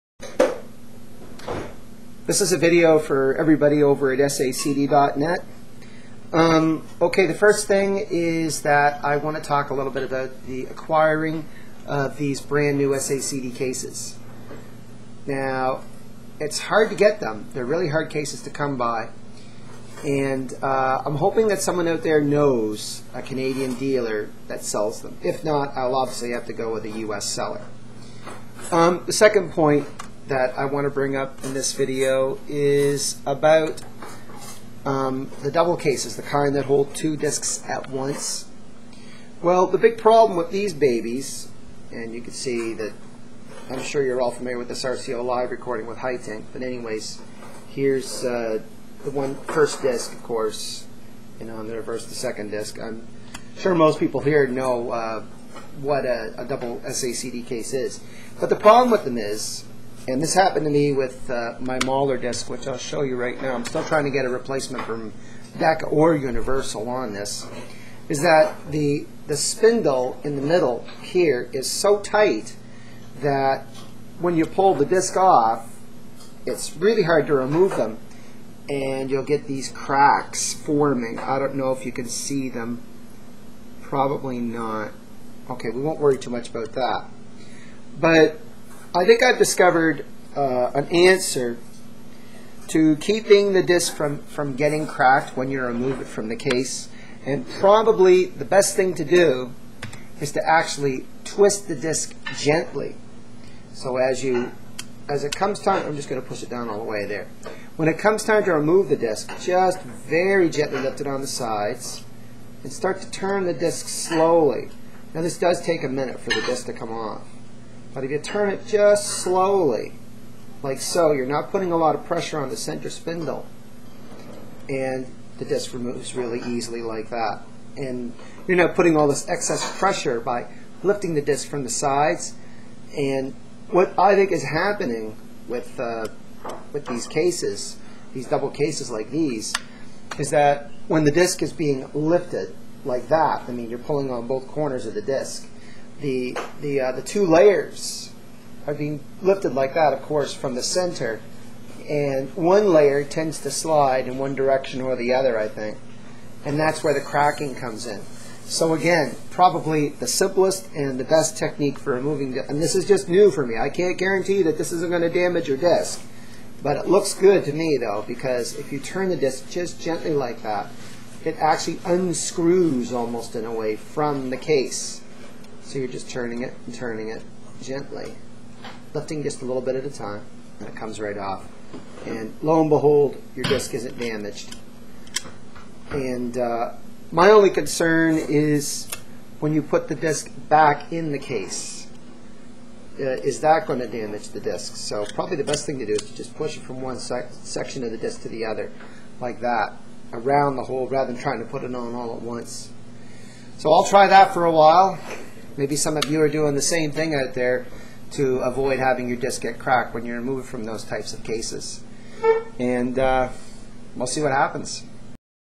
For SA-CD.net: Avoiding SA-CD Spindle Cracks
This brief video offers a simple suggestion on how to remove SA-CD's from very tight cases. I also would like to know if there are any dealers in Canada who sell the Super Jewel Box Cases that SA-CD's come in. Do you know a dealer? Please let me know.